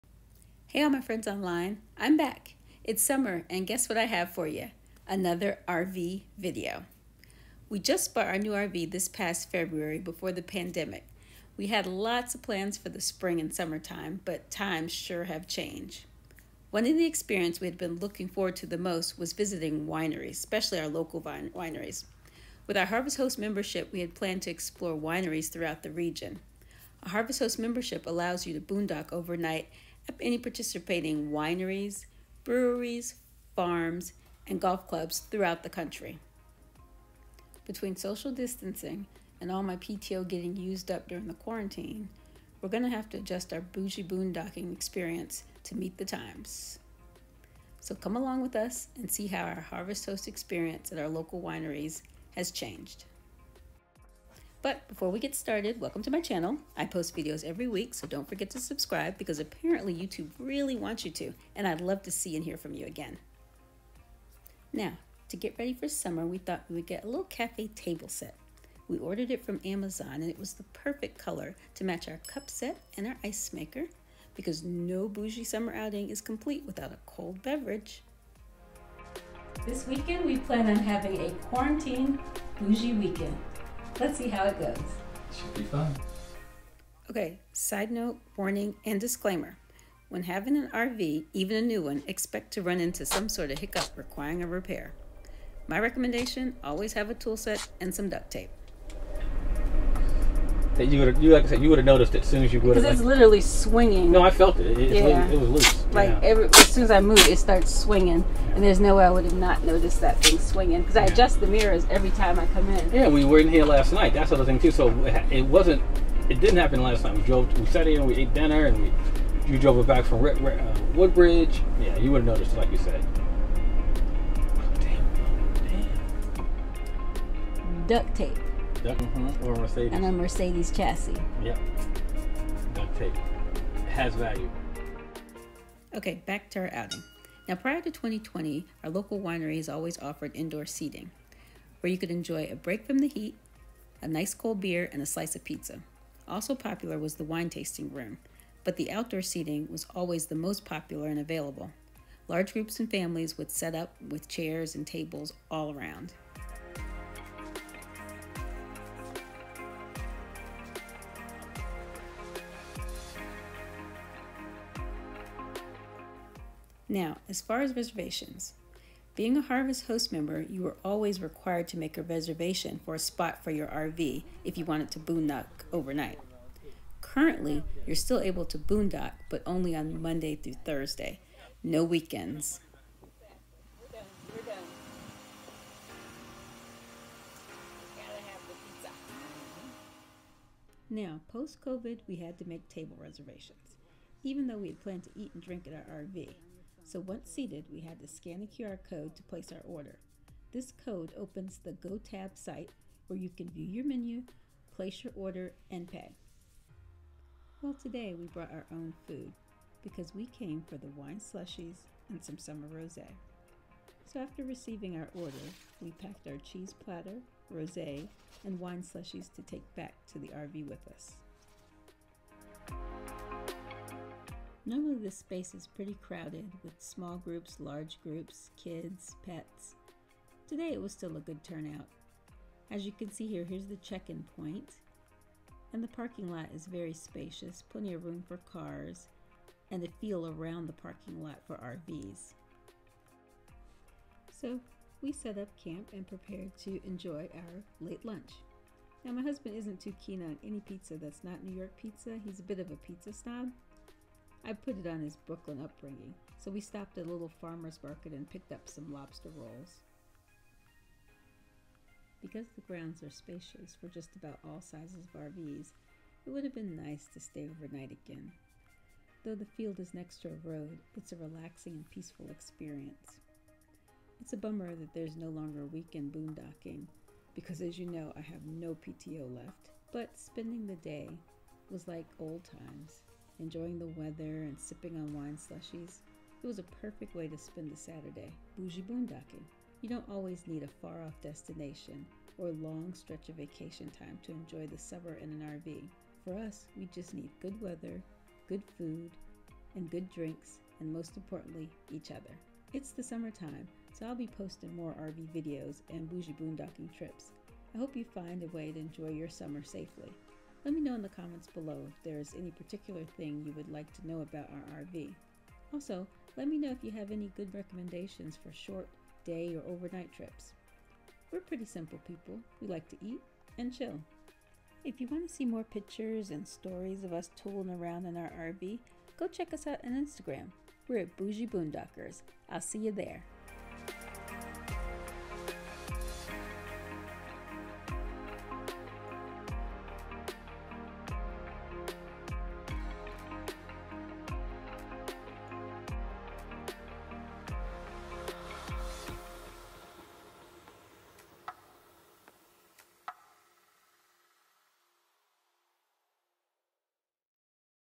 How to enjoy your RV during a pandemic | RV trips and social distancing.
Hey there all my friends on line I am back it is summer guess what I got, another RV video.

We just bought our new RV this past February Before the pandemic we had lots of big plans for the spring and summer. but times sure have changed.

One of the experiences we had been looking forward to the most was visiting wineries. With our harvest host membership  we had planned to explore wineries throughout the region. A harvest host membership allows you to boondock overnight at participating wineries, breweries, farms and golf clubs throughout the country.

Between social distancing and all my pto getting used up during the quarantine, we are going  to have to adjust our BOUGIEboondocking to meet the times. so come along with us and see how our HH experience at our local winery has changed.

I have so much more for you so click the notification button so you will know the moment I upload a new video.

If you would like to see more of me, you can follow me on